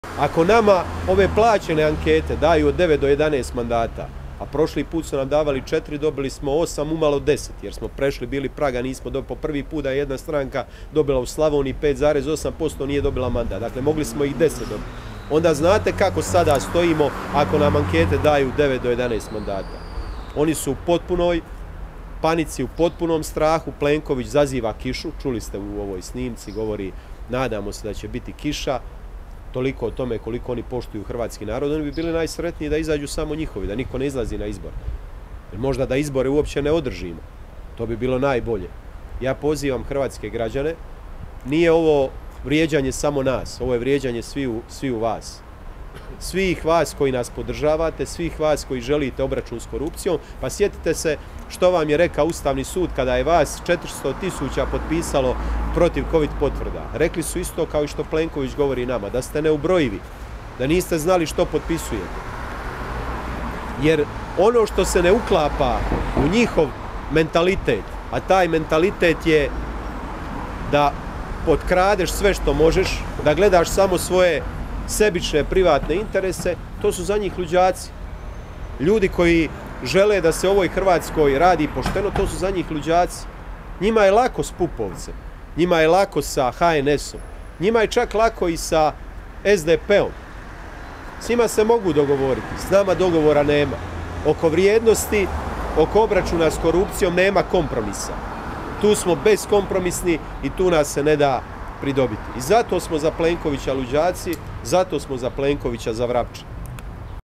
Grmoja o HDZ-u: "Sa SDP-om se mogu dogovoriti. S nama dogovora nema"
Zadnjeg dana izborne kampanje za parlamentarne izbore 2024. Nikola Grmoja je na konferenciji za medije održanoj ispred sjedišta DORH-a govorio o kaznenoj prijavi koju je upravo podnio protiv Pave Vujnovca, Fabrisa Peruška i drugih.